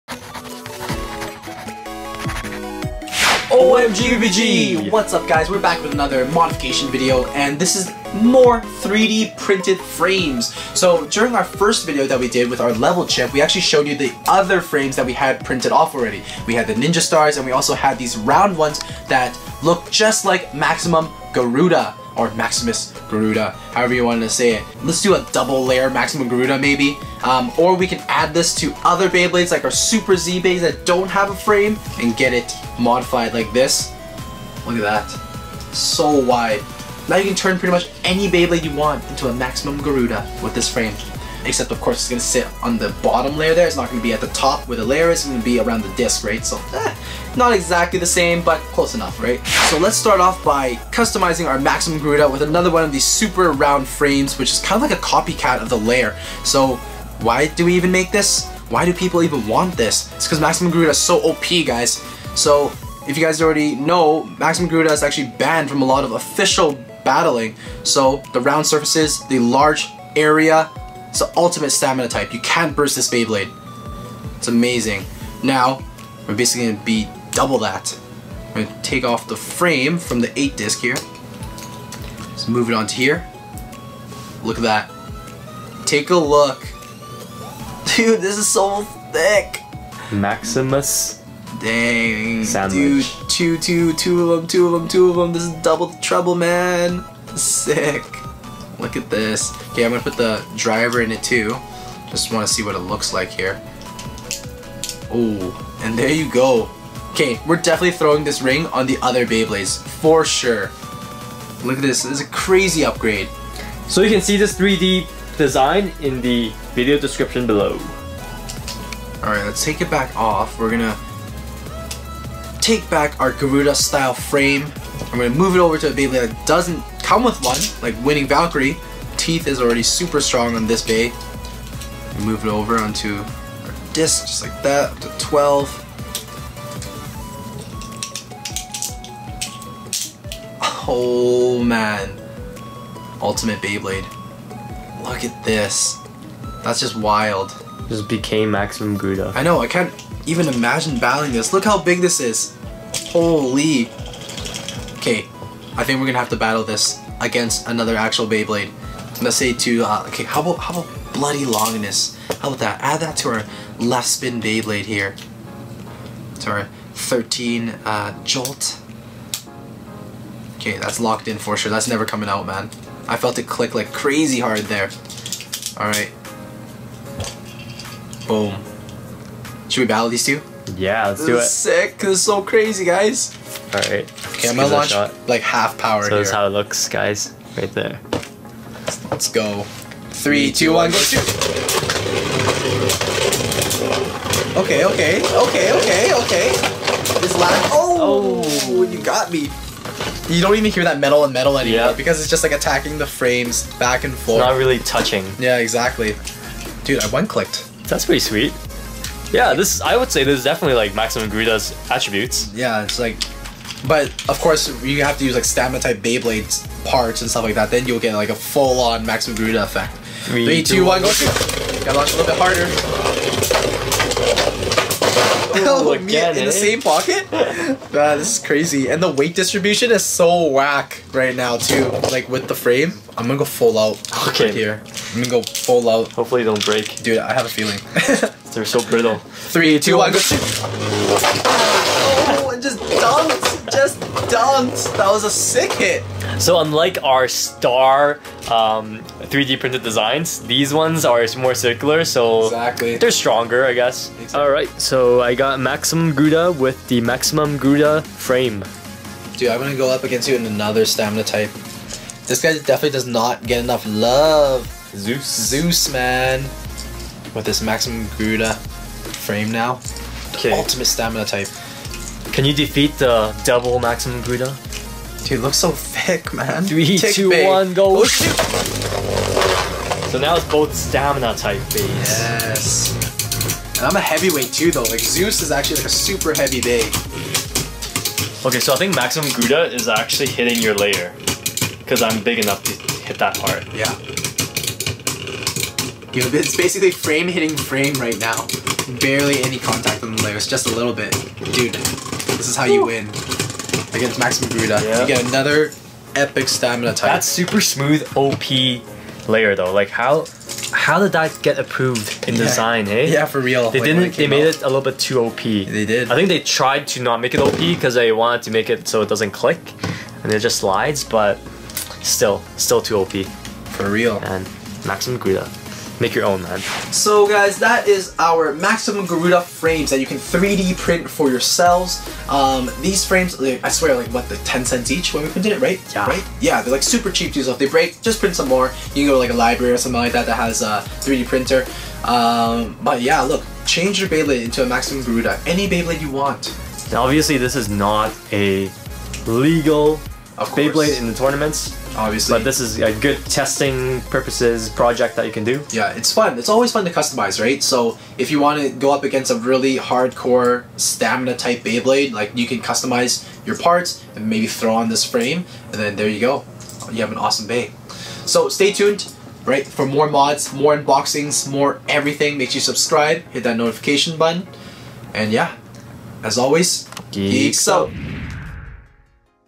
Make Any Beyblade like MAXIMUM GARUDA! - Epic Custom Beyblade Burst Frames
If you’ve watched our previous 3D printed Level Chip video, you probably say that we had other custom goodies! We already made a video on the ninja star frames and they weren’t that durable. This time, we’re showcasing these frames that are inspired by Maximum/Maximus Garuda! They’re meant to replicate the smooth, round surface of the Garuda Layer, increasing stamina and absorb hits. Of course, these are also prototypes and by no means OFFICIAL parts.

Whatcha guys think of these custom frames? Mad props to neosarchizo for making these amazing designs! Check out his designs below: